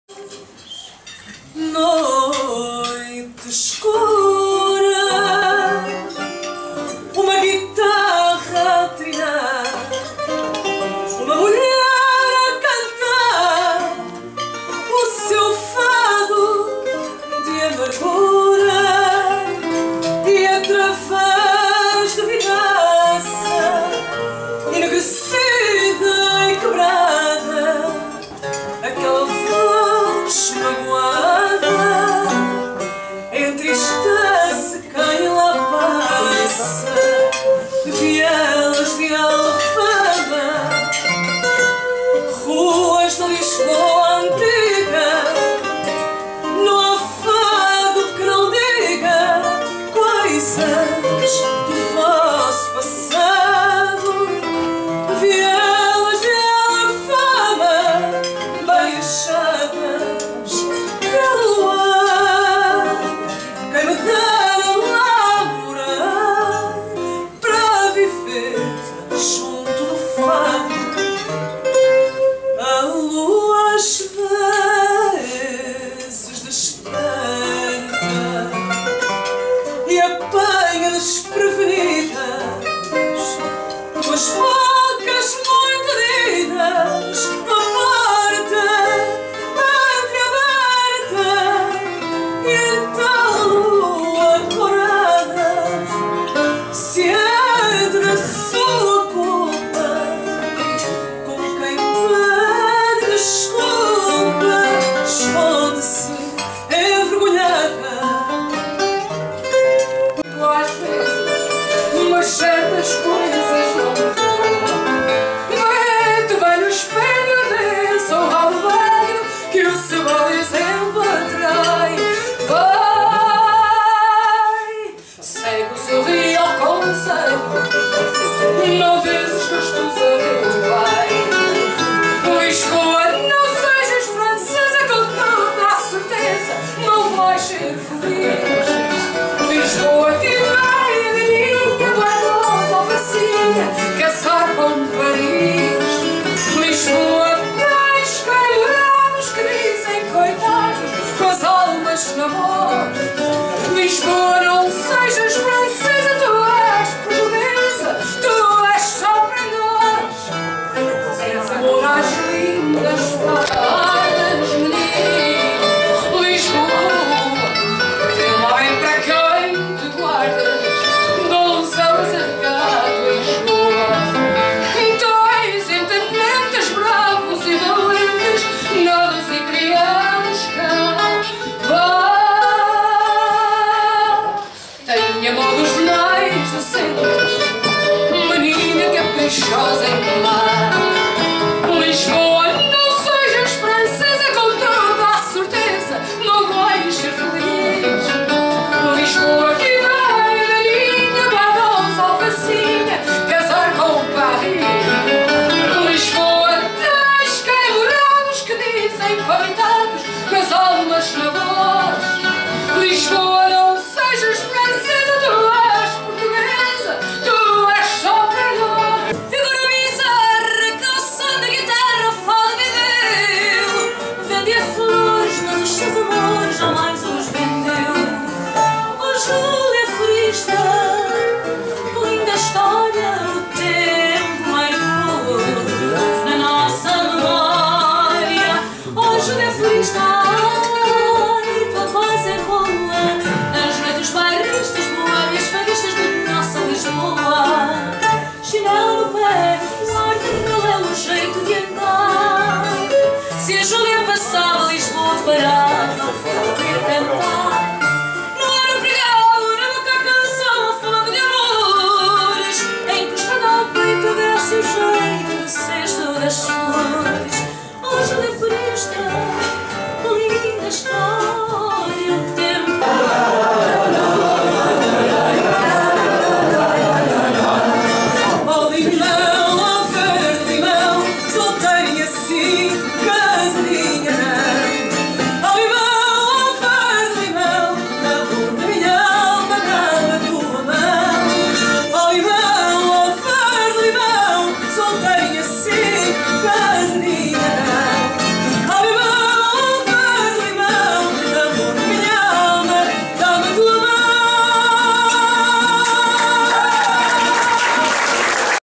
Lisbonne Portugal 2014
Balade à Lisbonne Portugal 2014
Images et son, montage Pascal Colombain
Musique:  Fado (extraits) enregistrements personnel  au restaurant le Guitarras à Lisbonne